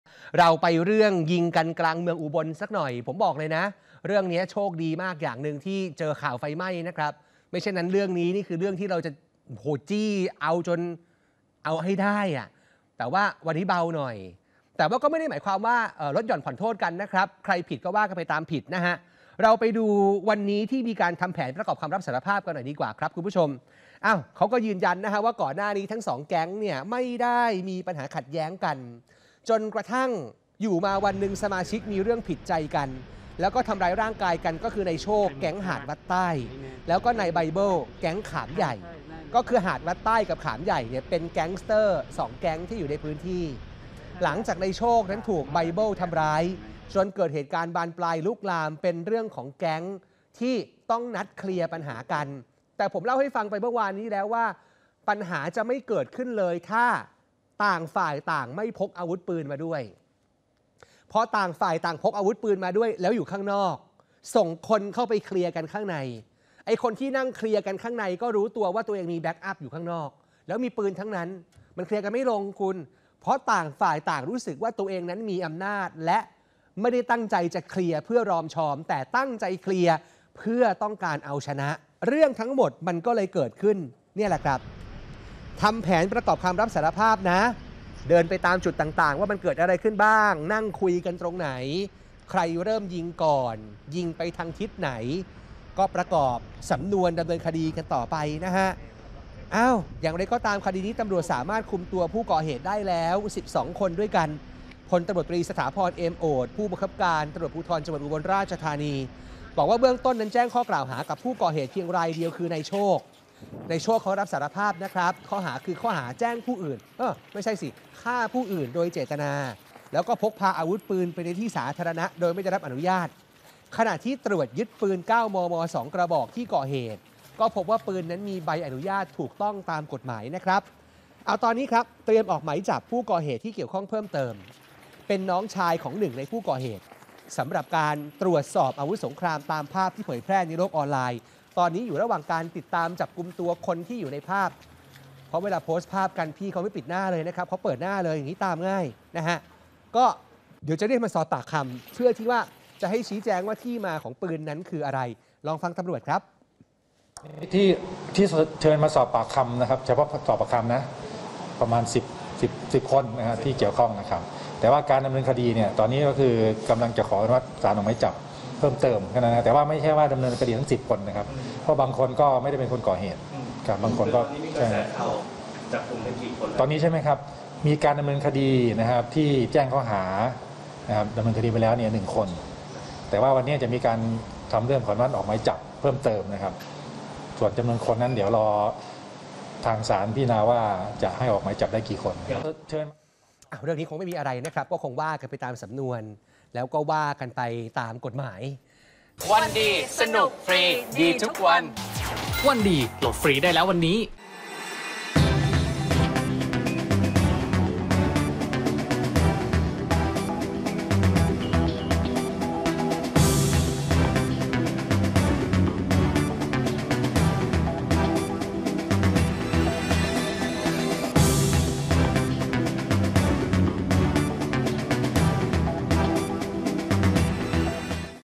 จ่อออกหมายจับเพิ่ม! โจ๋ดวลปืนกลางอุบลฯ | ข่าววันศุกร์ | ข่าวช่องวัน
เหตุวัยรุ่น 2 แก๊งซึ่งเป็นการ์ดร้านอาหารในเมืองอุบลราชธานีชักปืนดวลกันกว่า 100 นัด ตำรวจคุมตัวผู้ก่อเหตุได้แล้ว 12 คน และเตรียมออกมายจับเพิ่มอีก 1 คน และนี่เป็นหลักฐานสำคัญอีกชิ้นซึ่งยืนยันว่าใครเป็นผู้ก่อเหตุบ้าง